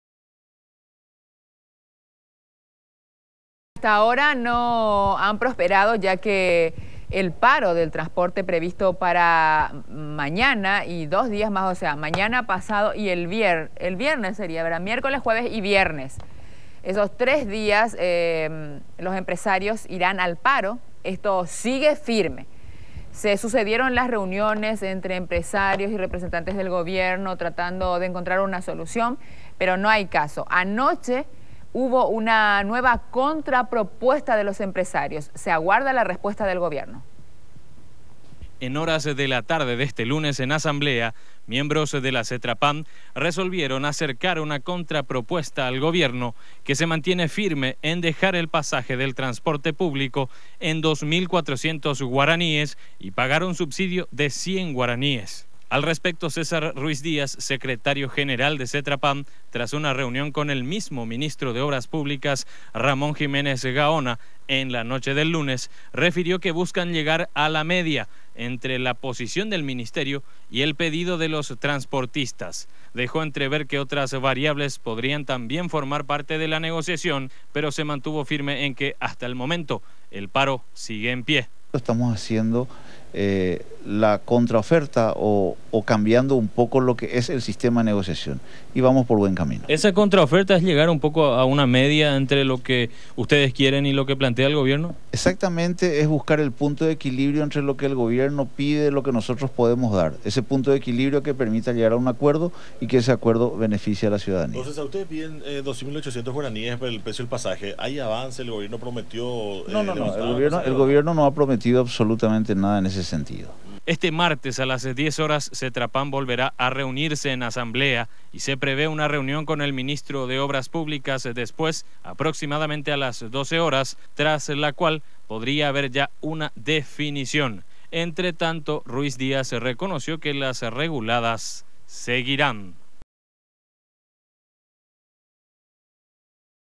Sigue en pie el paro de transportistas - 05/08/14
El titular de la Cetrapam, César Ruíz Díaz anunció el avance de negociaciones para evitar el paro de ómnibus, sin embargo la medida sigue en pie.